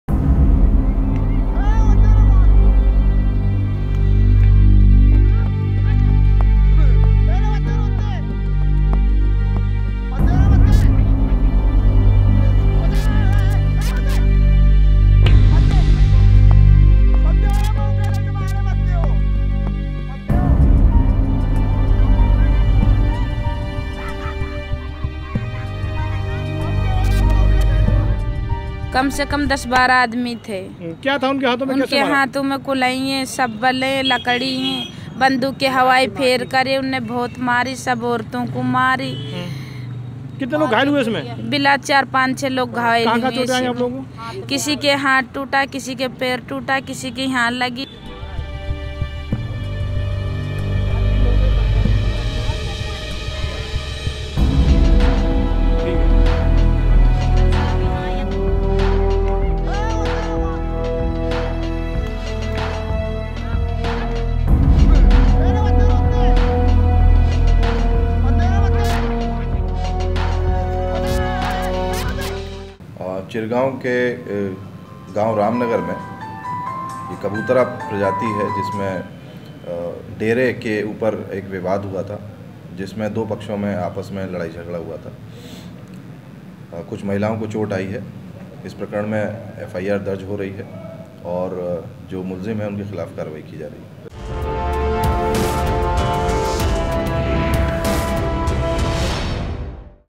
झांसी में महिलाओं को दौड़ा-दौड़ा कर पीटा | News Tak
झांसी में महिलाओं को दौड़ा-दौड़ा कर पीटा
झांसी जनपद में कबूतरा समुदाय के दो पक्षों में हुई जमकर मारपीट का वीडियो इन दिनों सोशल मीडिया पर वायरल हो रहा है. दरअसल डेरे पर वर्चस्व को लेकर दोनों पक्ष आपस में ही भीड़ गए. इसके बाद एक पक्ष ने दूसरे पक्ष की महिलाओं को दौड़ा-दौड़ा कर पीटा. महिलाओं के साथ मारपीट का यह वीडियो इन दिनों सोशल मीडिया पर वायरल हो रहा है. वहीं वायरल हो रहे वीडियो का संज्ञान लेते हुए एसएसपी ने कहा कि मामले में आरोपियों के खिलाफ कड़ी कार्रवाई की जाएगी. उधर पीड़ित महिलाओं ने चिरगांव थाने की पुलिस पर ही गंभीर आरोप लगाए हैं.